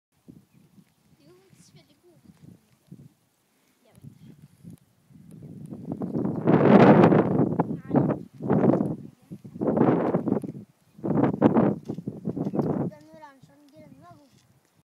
On the edge @ Himmelvann
View down towards Holmsbu, Dramsfjorden and Verket. Best view in Hurum